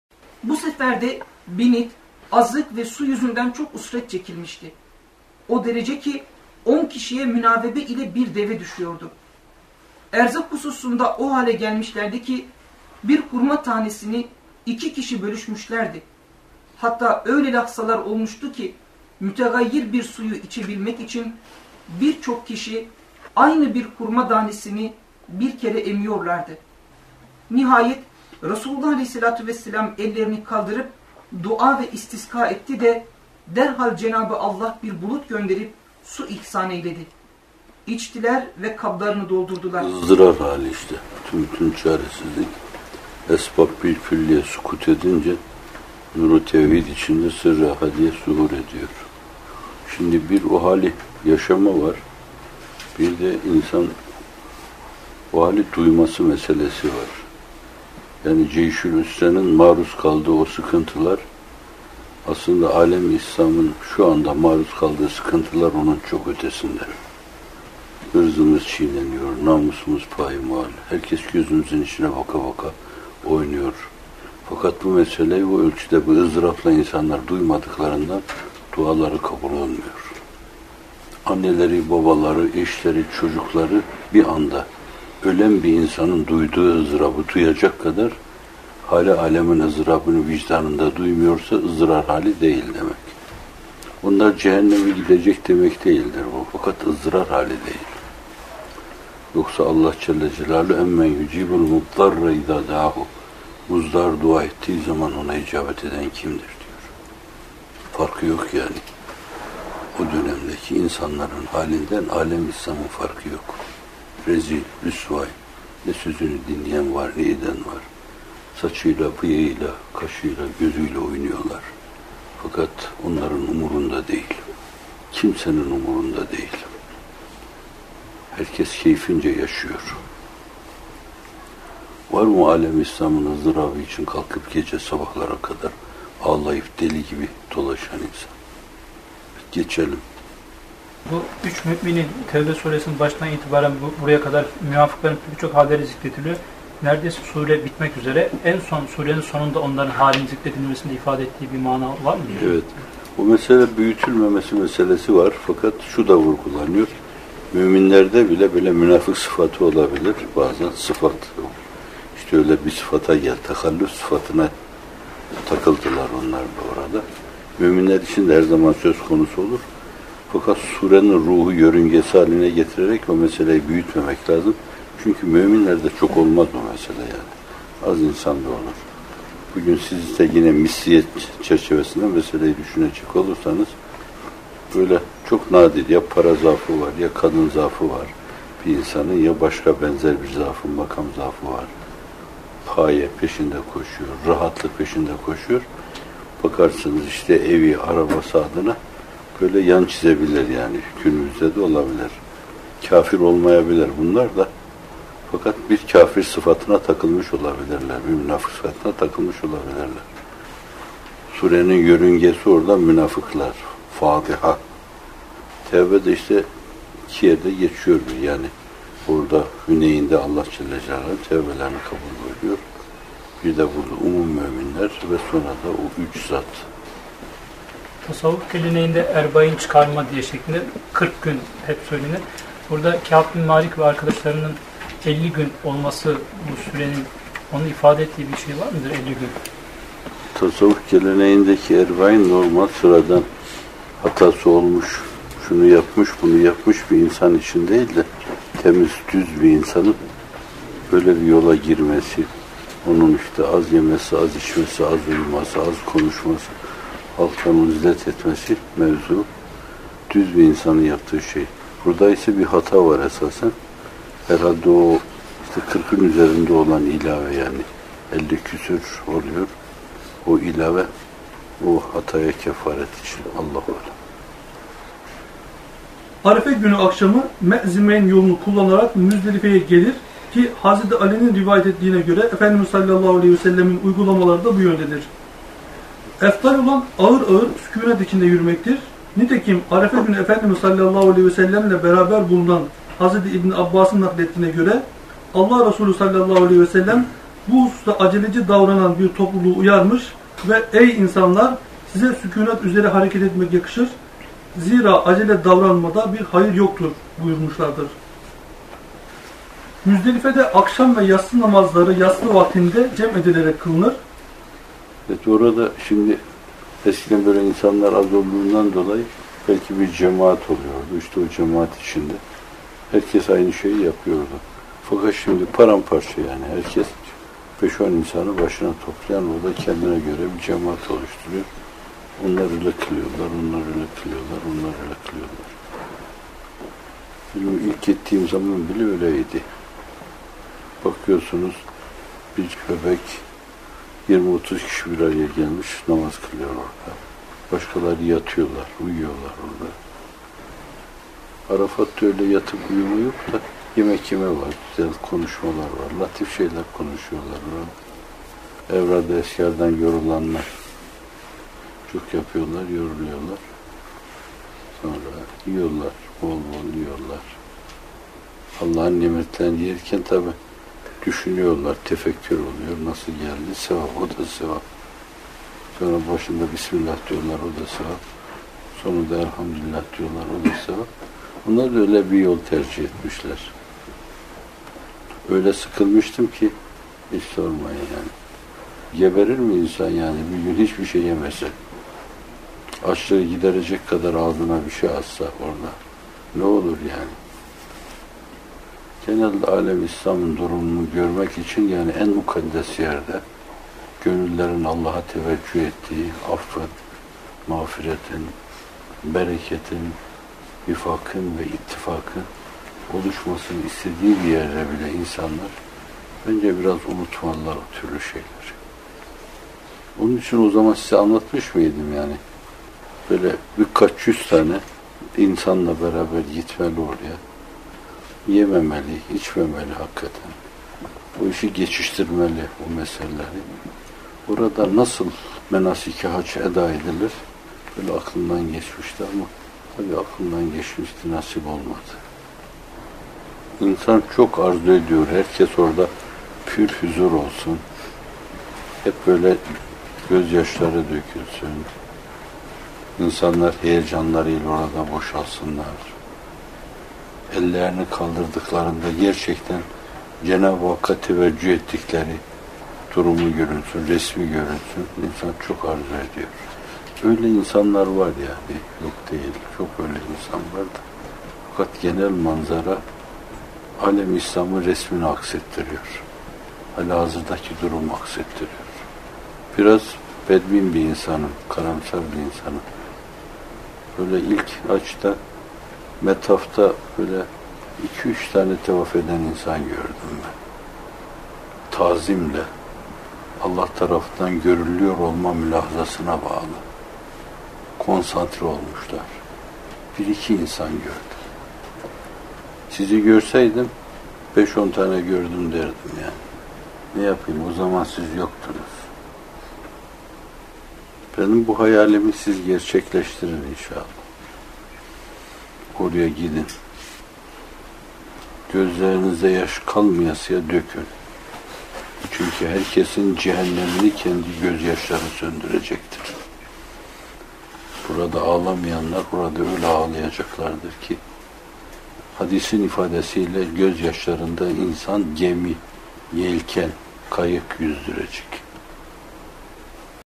Nağme - 11 - | Ceyşu’l-usret ve Arafat - Müzdelife Vakfeleri | M. Fethullah Gülen | (2012/04/22)
Tevbe Sûresi 117. ve 118. ayetlerin tefsirlerinden birkaç nükte ile fıkıh dersindeki açıklamalardan oluşan 9 dakikalık bu ses kaydında şu soruların cevaplarını bulacaksınız:

Tebük Seferi’nde Ceyşu’l-usret (zorluk ordusu) ne haldeydi?

Sefere çıkmayıp geride kalan üç sâdık mü’min ve tevbelerinin bu sûrede anlatılmasının hikmeti nedir?

O üç kişinin bekleme süreleri olan elli küsur gün ile “erbain” arasındaki en büyük fark adına neler söylenebilir?

Arafat ve Müzdelife vakfelerini mü’minler nasıl değerlendirmelidir?